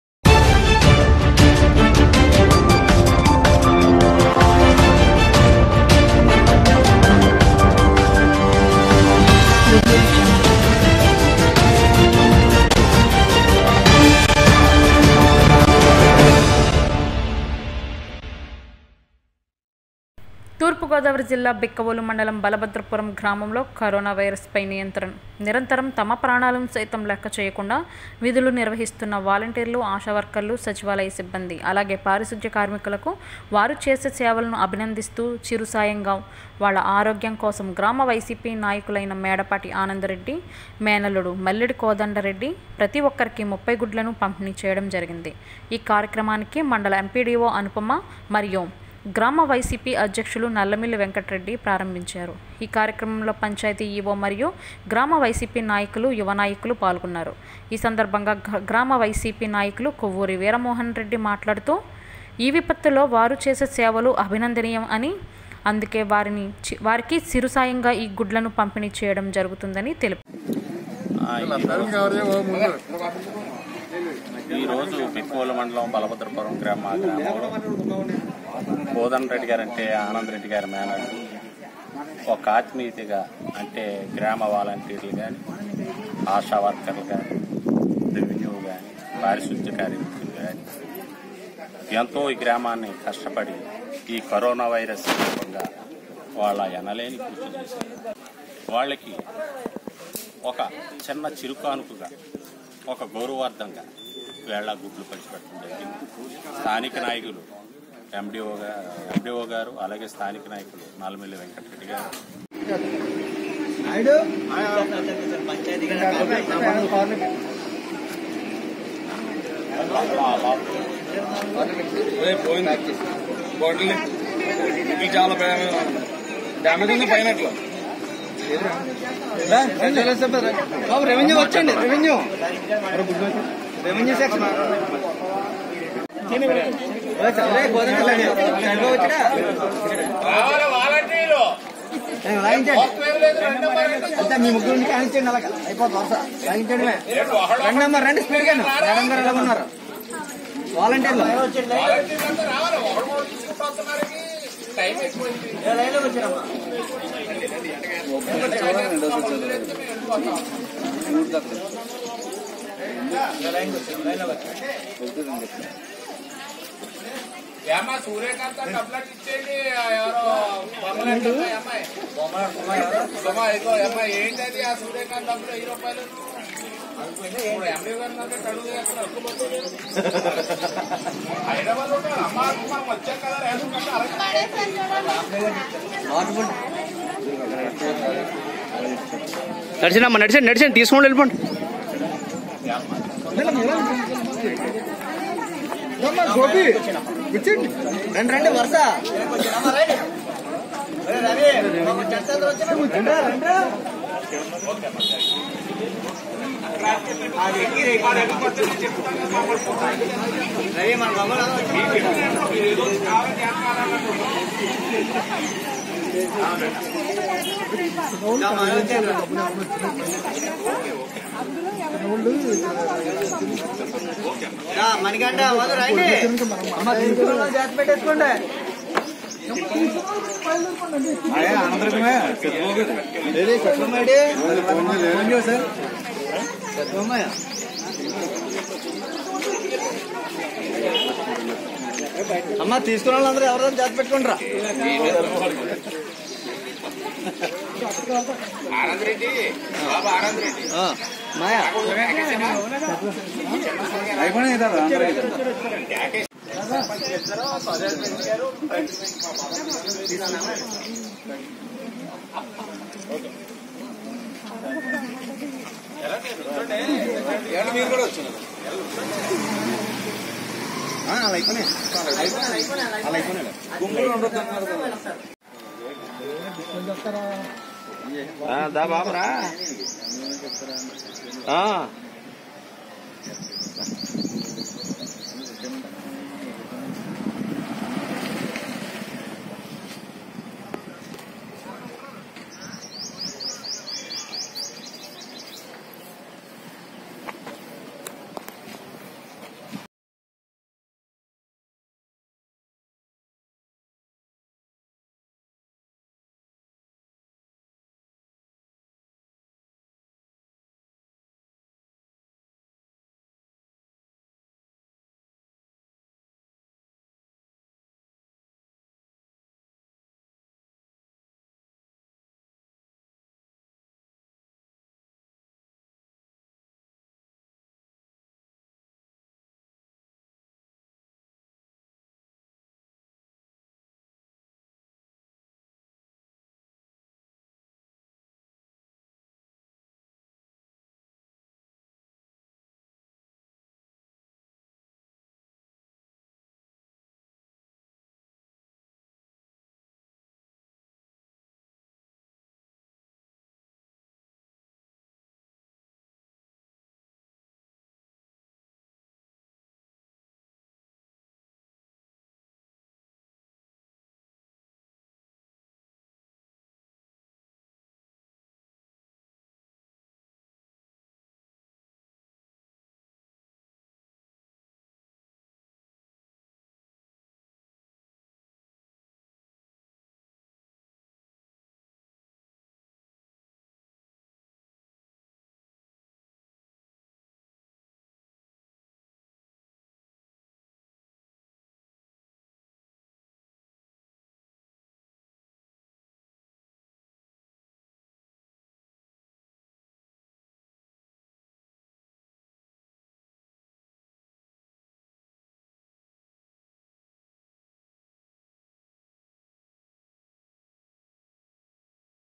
బిక్కవోలు మండలం బలభద్రపురం గ్రామంలో కరోనా వైరస్ పై నిరంతరం తమ ప్రాణాలును సైతం లెక్కచేయకుండా
బిక్కవోలు మండలం బలభద్రపురం గ్రామంలో కరోనా వైరస్ పై నిరంతరం తమ ప్రాణాలును సైతం లెక్కచేయకుండా విధులు నిర్వహిస్తున్న వాలంటీర్లు,ఆశావర్కర్లు,సచివాలయ సిబ్బంది,అలాగే పారిశుద్యకార్మికులుకు